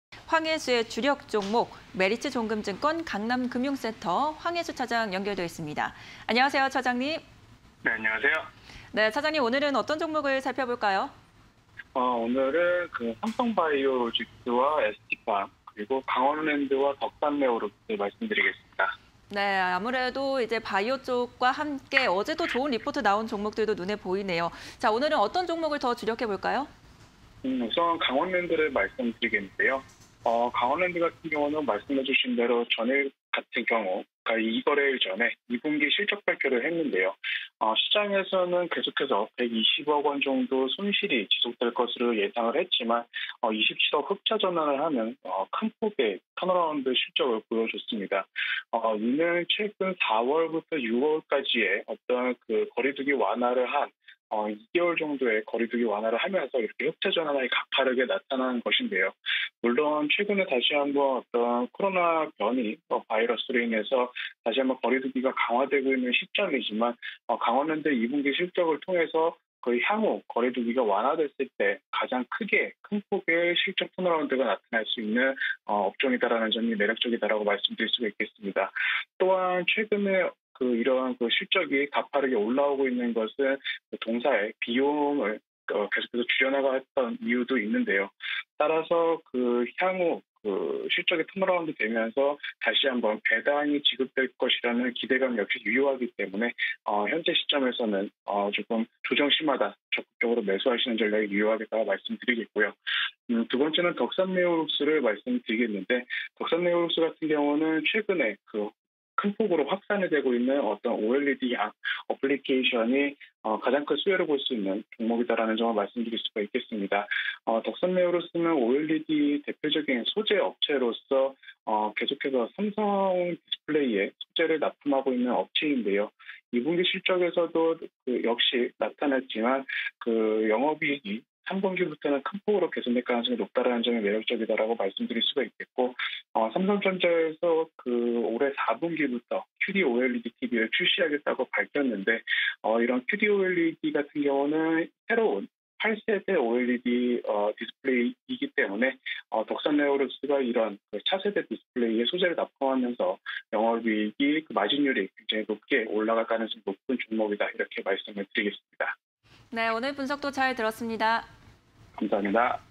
강원랜드, 덕산네오룩스, 삼성바이오로직스, 에스티팜_황해수의 주력 종목 (20210812)
1.삼성바이오로직스(207940)
2.에스티팜(237690)
3.강원랜드(035250)
4.덕산네오룩스(213420)

강원랜드(035250)
- 2분기 어닝 서프라이즈 기록
- 거리두기 완화 시 가파른 이익 성장 전망
- 이익률 회복과 배당 지급 기대감 증가

덕산네오룩스(213420)
- 삼성디스플레이 신규 디스플레이 개발 수혜주
- 2022년부터 높은 이익 성장 예상
- OLED 시장 확장에 의한 중장기 상승 사이클 기대